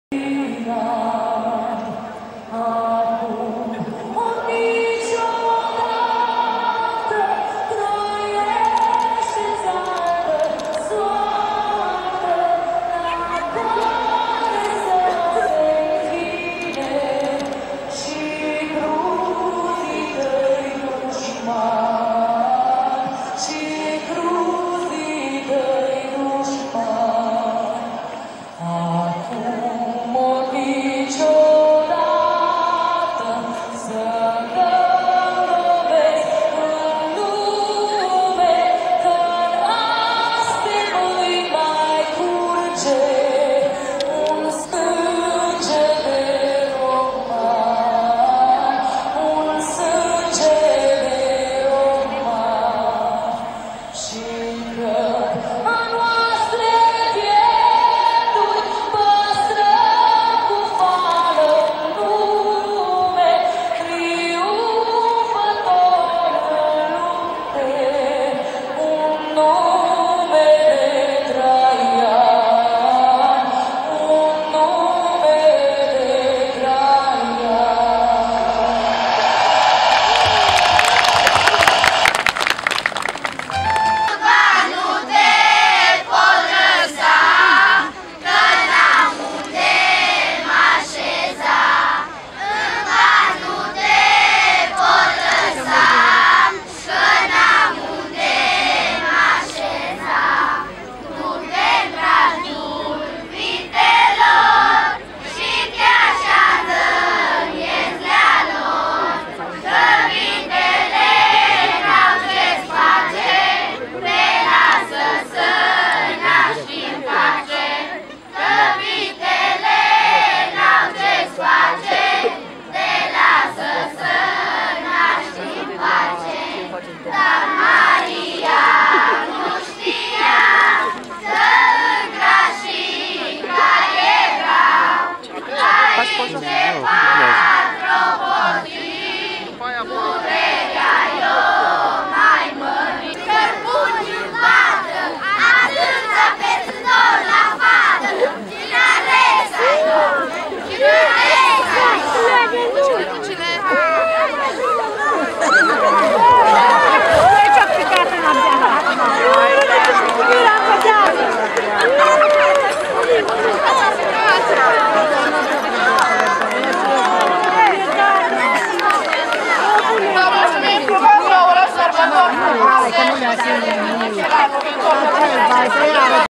Colinde din Giulesti de Sf. Nicolae 6 Decembrie 2012 .mp4
FIlmari Evenimente in HD o filmare cu aparat Foto Nikon D7000